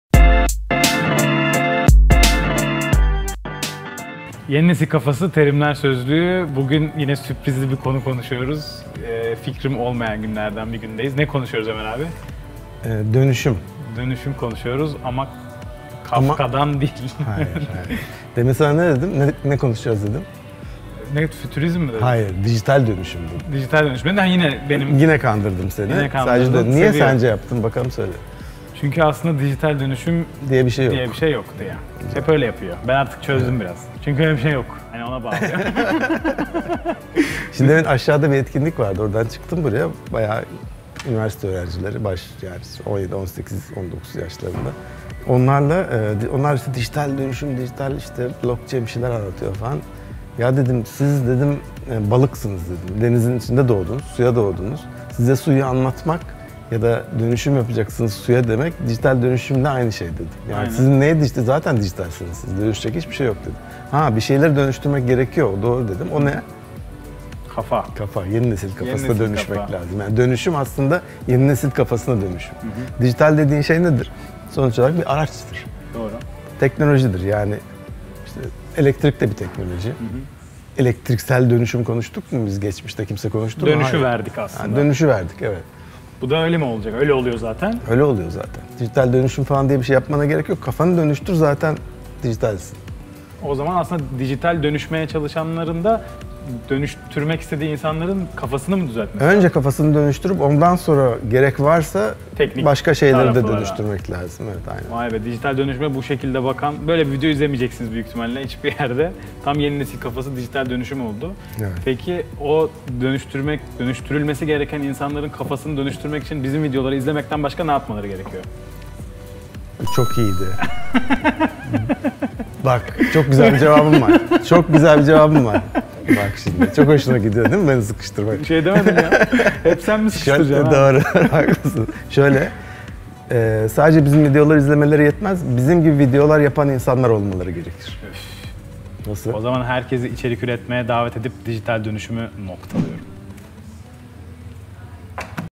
Girişimcilik - “Dönüşüm” | Terimler Sözlüğü - Temel Kavramlar
Ömer Abi, Sonat'ı yine kandırıyor. Dijital dönüşüm konuşacaklarını düşünürken, tekrar tek bir başlıkta topluyor Ömer Erkmen.

Yeni nesil kafada değişmek isteyen, dönüşmek isteyen şirket ve ekiplerin, bizi izlemekten başka ne yapması gerektiğini konuştuk. Cevabı videoda. Gerisi sizin yorumlarınızda.

Yorum bırakmayı ve videoyu beğenmeyi unutmayın.

Videolar önce Linkedin'de daha sonra her yerde.

Girişimcilikte temel kavramlar
Girişimcilik terimleri